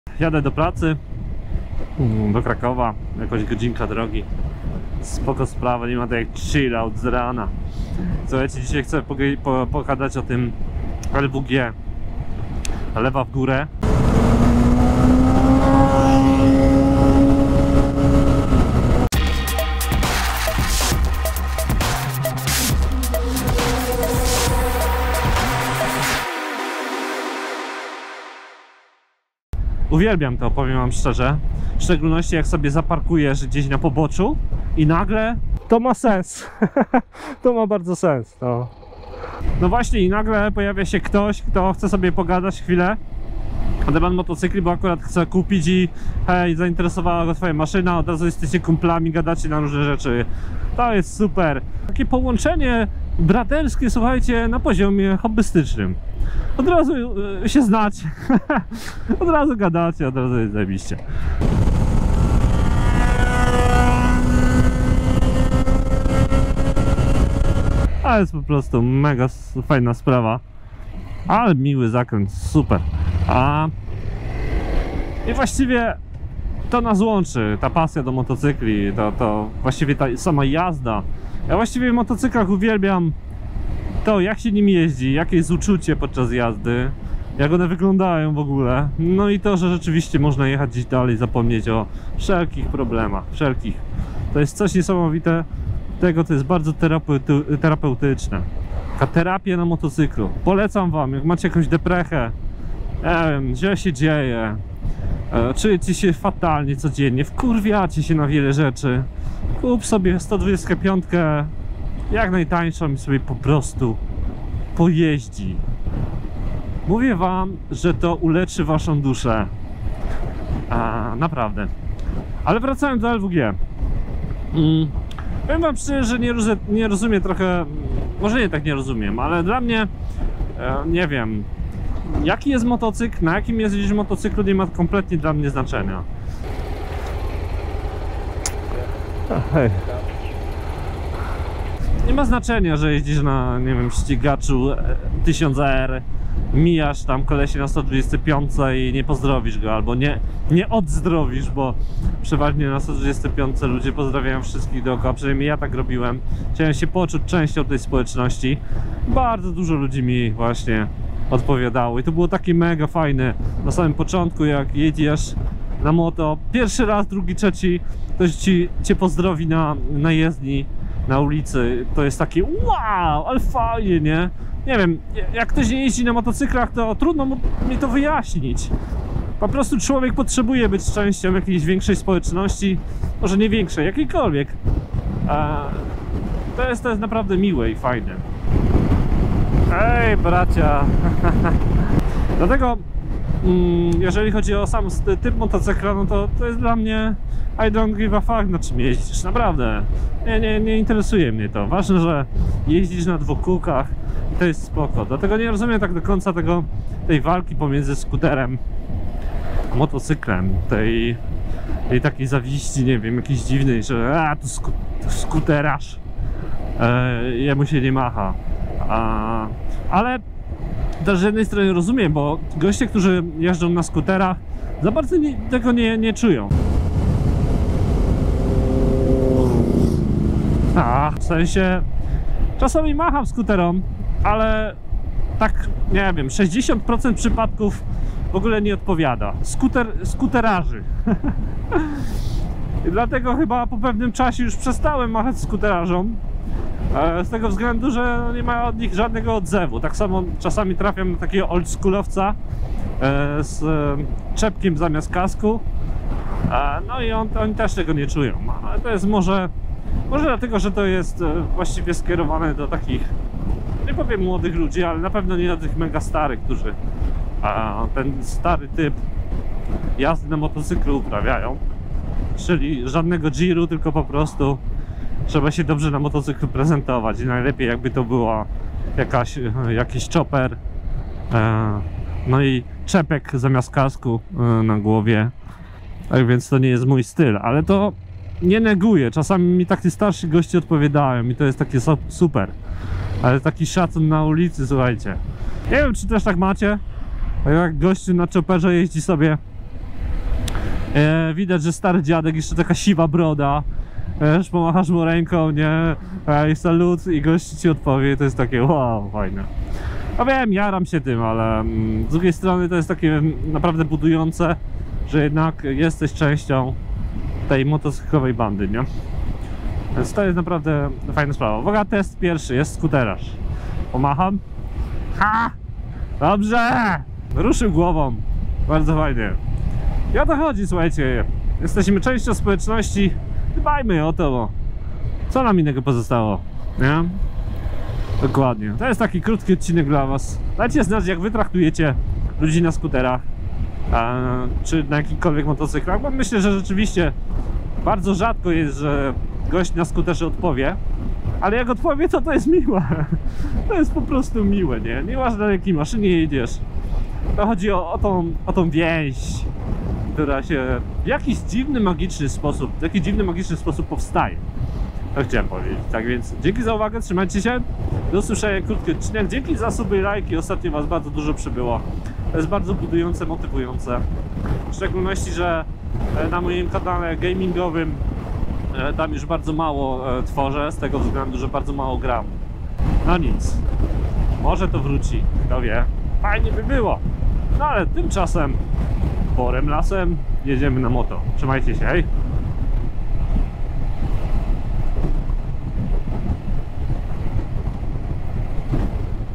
Co z tym LWG? Machasz skuterom? | Przegląd mijanych motocykli
LWG to dobra strona tego hobby. Wiele osób nie rozumie o co chodzi, zatem śpieszę z wyjaśnieniem. Swoją drogą to jak reagujecie na osoby jeżdzące na skuterach?

Zostaw suba, lajka i może podeśli ten film znajomemu, jeśli przypadł Ci do gustu ;)